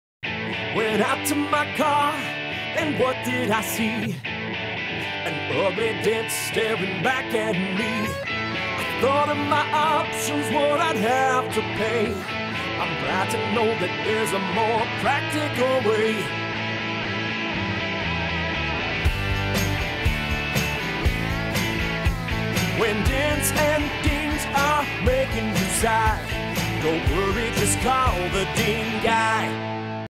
DingGuyMusicVideo
30 second commercial featuring the band GhostRADIO